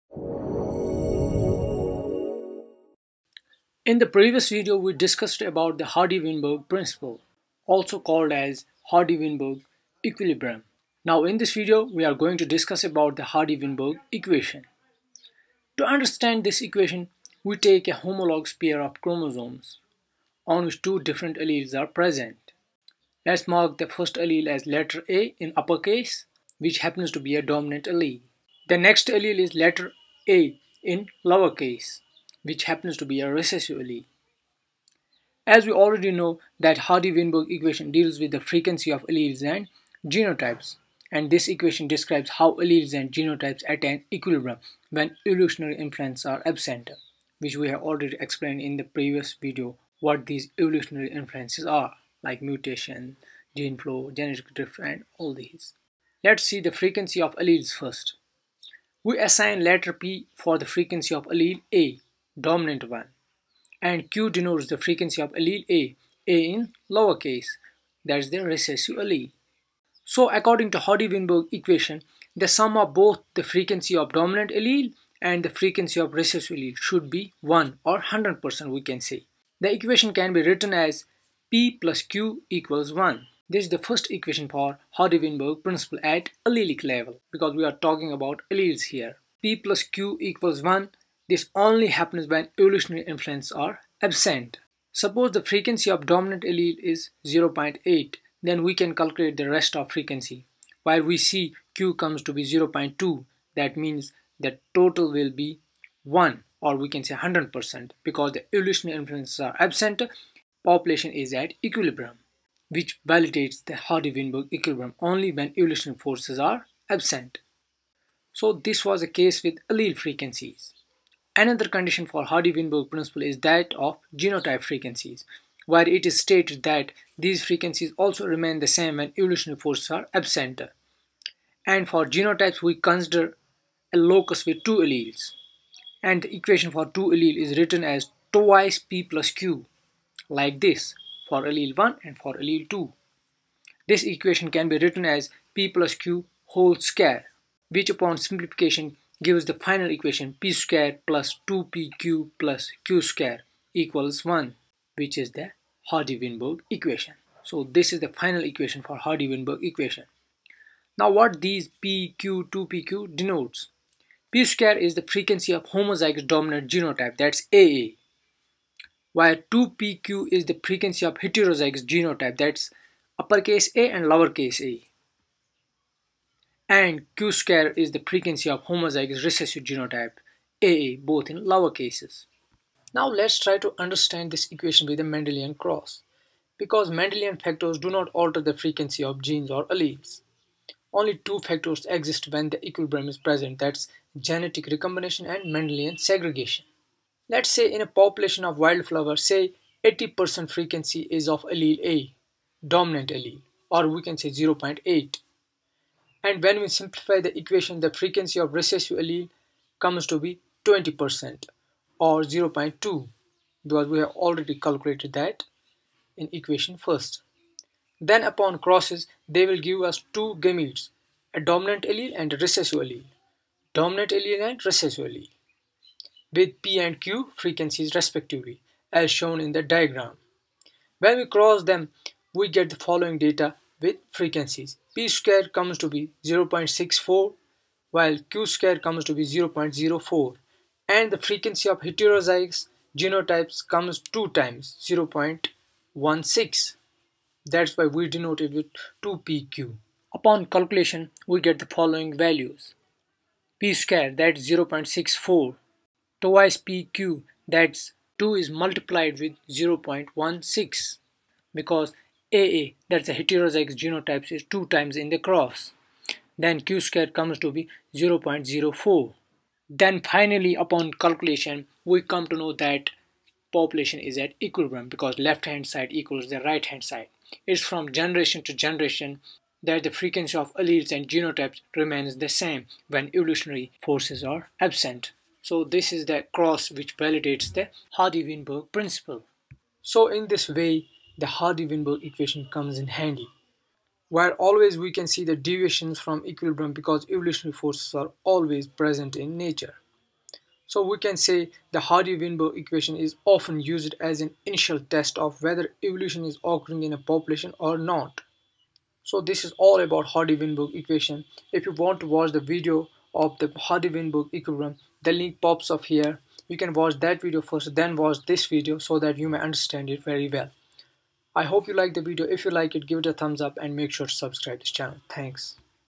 Hardy-Weinberg Equation | Detailed
The Hardy–Weinberg principle, also known as the Hardy–Weinberg equilibrium, model, theorem, or law, states that allele and genotype frequencies in a population will remain constant from generation to generation in the absence of other evolutionary influences. These influences include mate choice, mutation, selection, genetic drift, gene flow and meiotic drive.
Consider a population of monoecious diploids, where each organism produces male and female gametes at equal frequency, and has two alleles at each gene locus. Organisms reproduce by random union of gametes (the “gene pool” population model). A locus in this population has two alleles, A and a, that occur with initial frequencies f0(A) = p and f0(a) = q, respectively.[1] The allele frequencies at each generation are obtained by pooling together the alleles from each genotype of the same generation according to the expected contribution from the homozygote and heterozygote genotypes.
The Hardy Weinberg equilibrium comes to be p+q=1